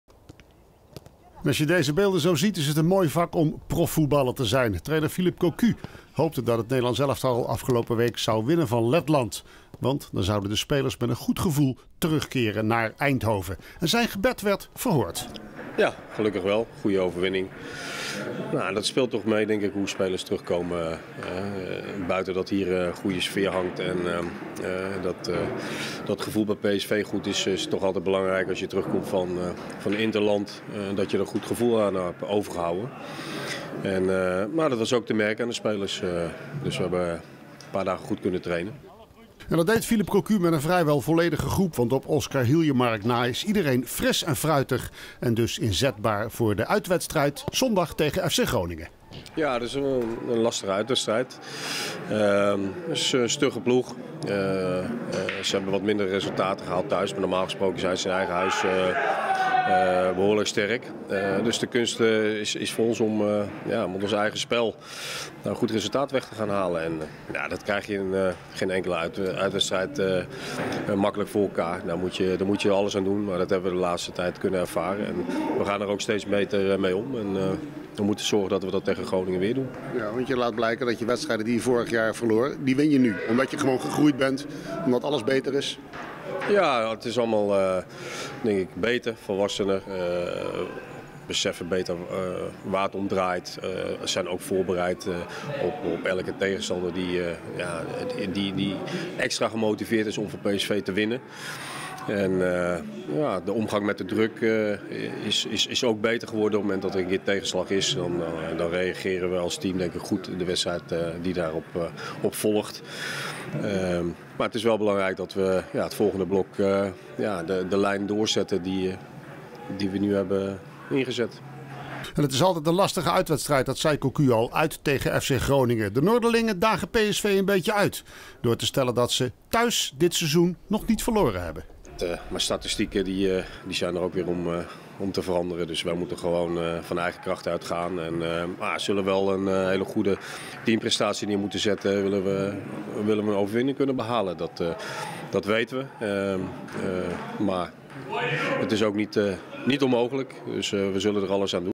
Cocu noemt FC Groningen-uit lastige wedstrijd
PSV won voor het laatst in 2012 in Groningen, dat zelf dit seizoen thuis nog niet verloor.